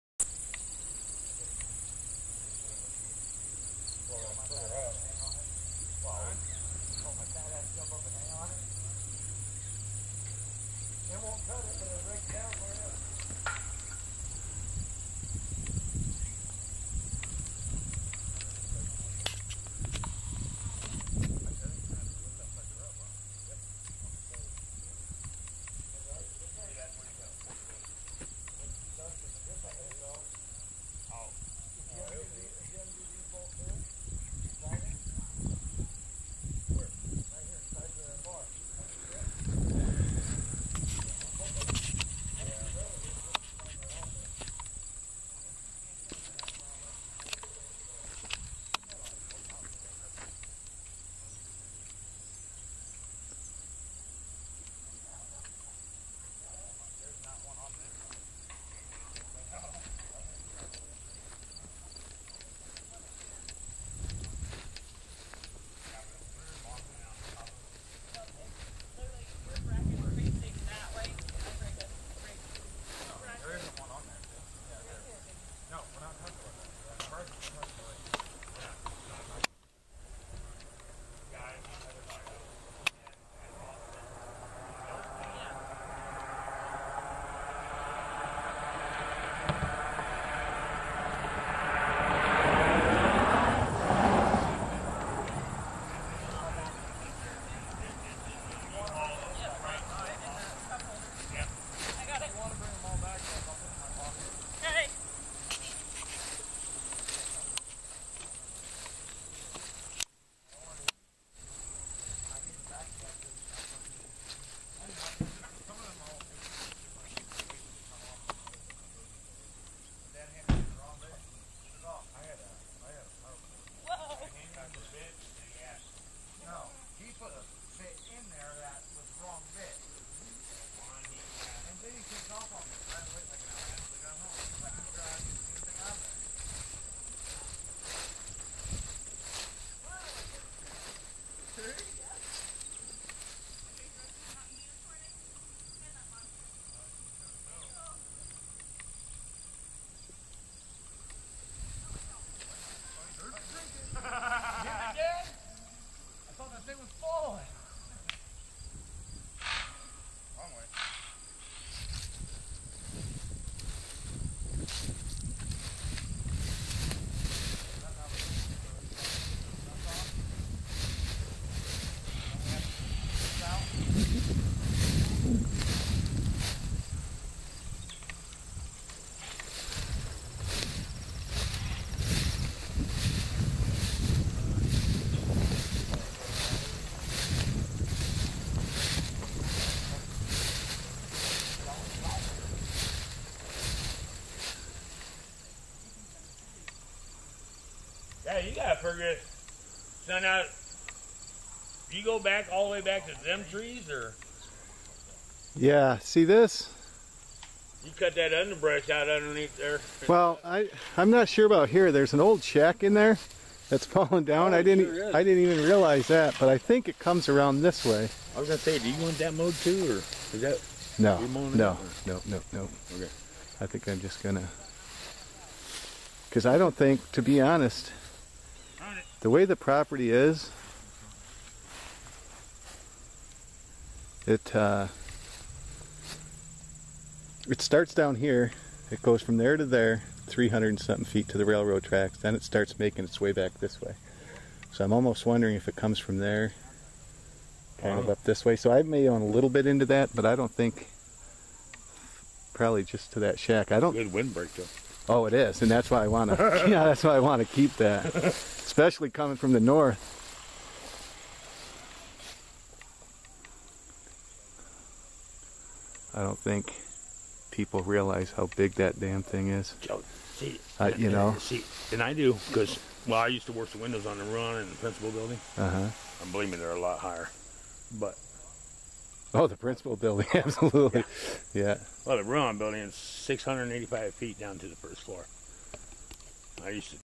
1.5 Acre Property
Vacant land in Southern Iowa that I will build my debt-free cabin on over the next few years. Property clearing began today which includes removing a 105' antenna tower, 5 large satellite dishes and a shed that housed radio equipment.